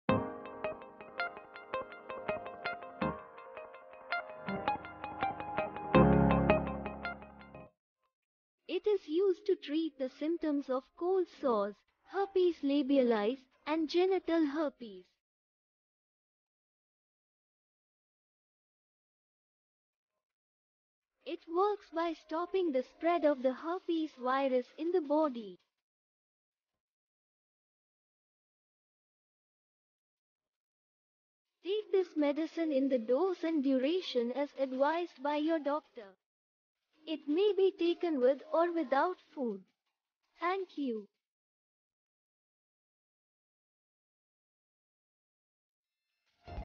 Cycloex 800mg Tablet - Uses, Benefits and Side Effects
Cycloex 800mg Tablet
Composition- Acyclovir (800mg)

Introduction
It is used to treat the symptoms of cold sores (Herpes Labialis) and Genital Herpes.
It works by stopping the spread of the herpes virus in the body.
Take this medicine in the dose and duration as advised by your doctor. It may be taken with or without food.